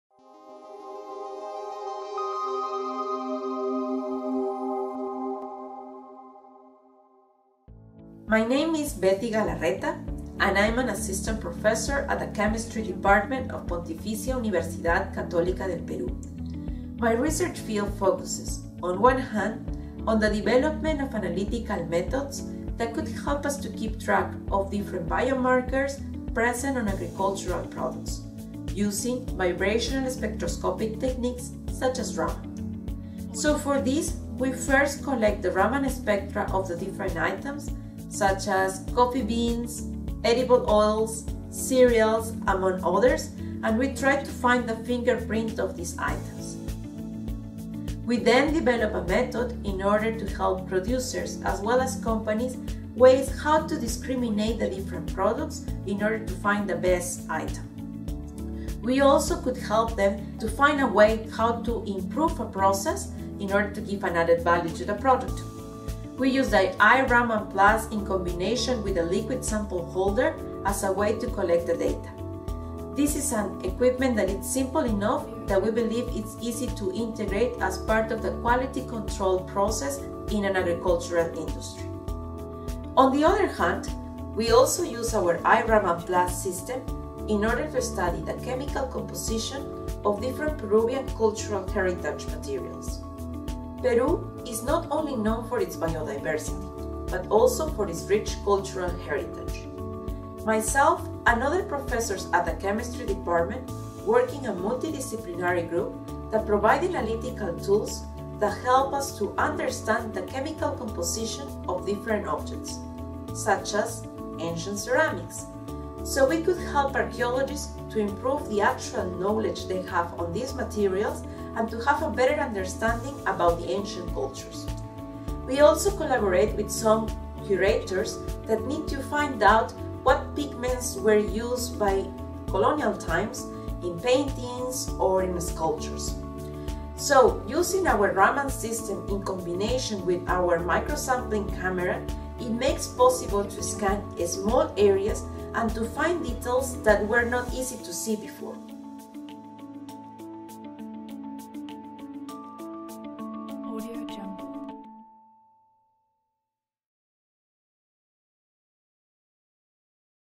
Testimonial - Portable Raman for Agricultural Products & Cultural Heritage in Peru
In this testimonial, we talk with Assistant Professor Betty Galarreta from the Pontificia Universidad Catolic del Peru. Betty's work using B&W Tek's i-Raman Plus Portable Raman Spectrometer involves developing analytical techniques that help track biomarkers within the Agricultural Industry. She also uses the i-Raman Plus to study the chemical composition of different Peruvian cultural heritage materials.